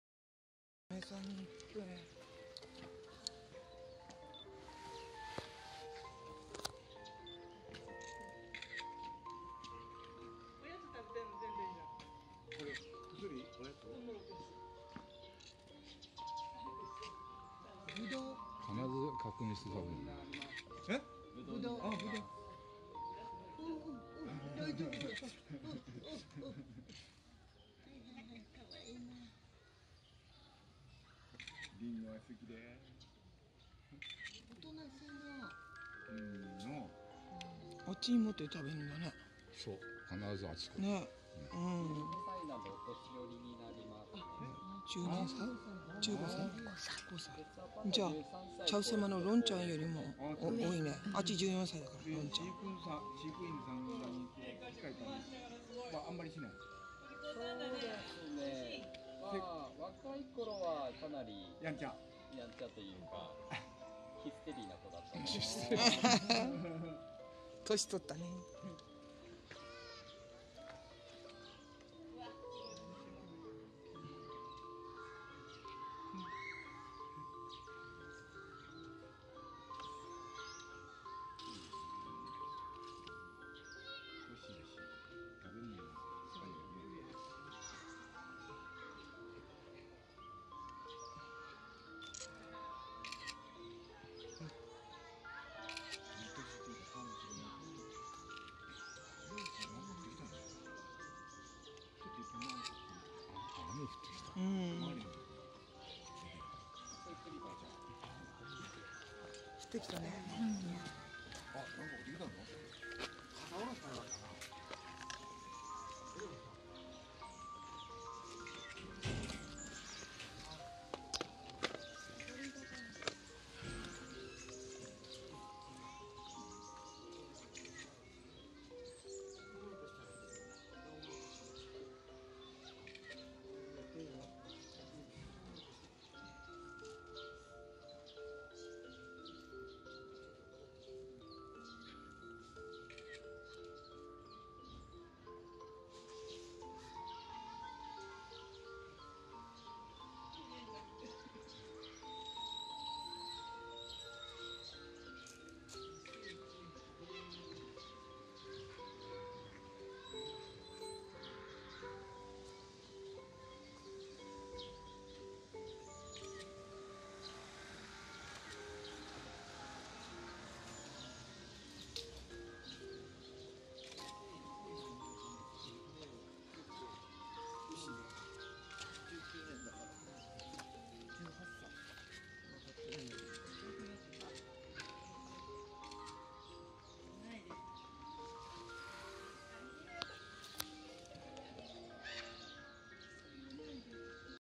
2019.10.3 11:36 釧路市動物園 レッサーパンダ コーアイ 🍇パクパクタイム🍎
この日は、好物の🍇をすんなり受け取って食べました！
15歳のコーアイちゃん。
飼育員さんから、「若い頃はかなりヒステリーなコでした」とありました😅
最近は、最後の🍎をもらう時に立たなくなってるけど、👀も歯も健在で元気なコーアイちゃん、ずっと健やかに暮らして欲しいです❤